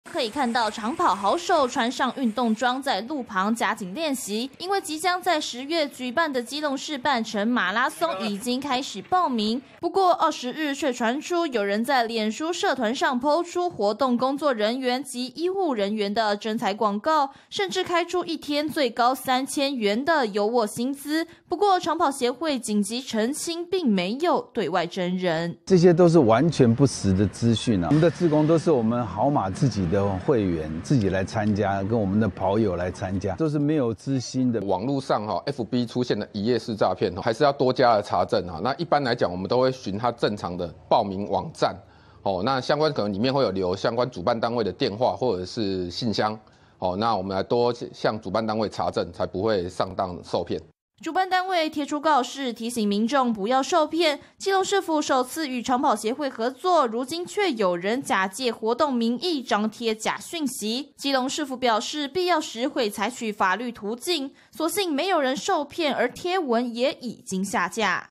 注意！ 基隆半程馬拉松「有詐」 詐團開日薪3千元徵才－民視新聞
【民視即時新聞】詐騙手法千百種，近日基隆就發生一起疑似詐騙的案件！近日有人假借基隆市半程馬拉松活動的名義，在臉書貼出徵才廣告，甚至開出日薪3千的高薪，引發熱議，詳情連線記者鄧庭婷。